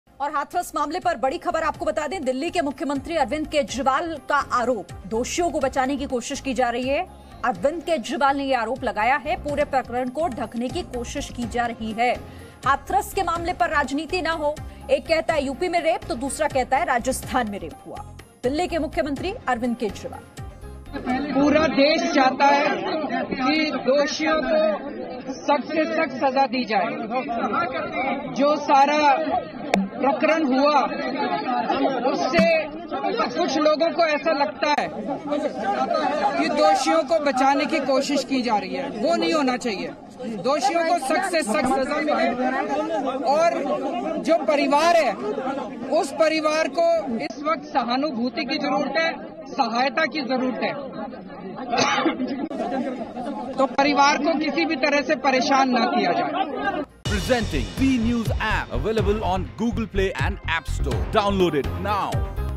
Hathras Case पर बोले Delhi CM Arvind Kejriwal: दोषियों को बचाने की कोशिश की जा रही है
The incident of Hathras gang rape is now taking political color. Delhi Chief Minister Arvind Kejriwal has slammed the government of Uttar Pradesh fiercely.

हाथरस गैंगरेप की घटना अब सियासी रंग लेती जा रही है. दिल्ली के मुख्यमंत्री अरविंद केजरीवाल ने उत्तर प्रदेश की सरकार पर जमकर हमला बोला है.